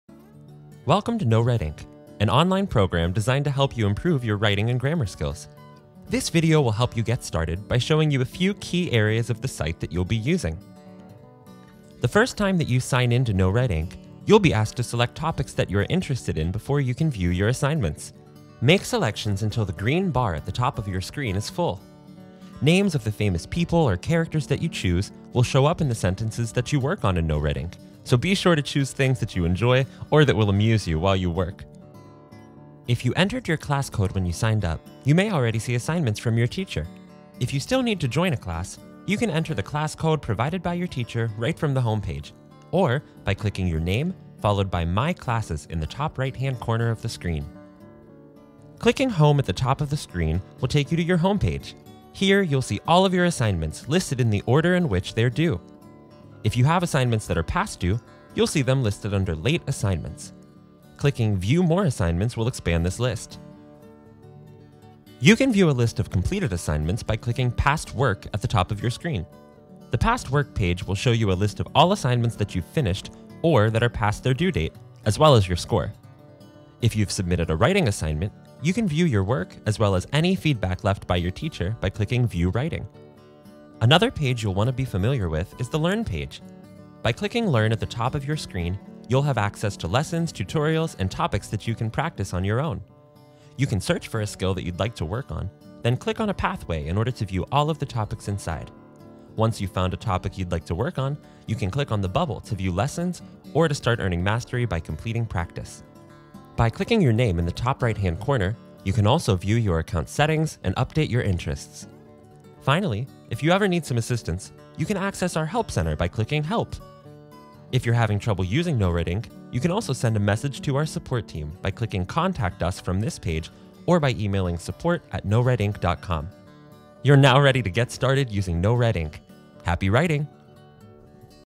Students: Get Started Using NoRedInk
A video tour to introduce students to NoRedInk.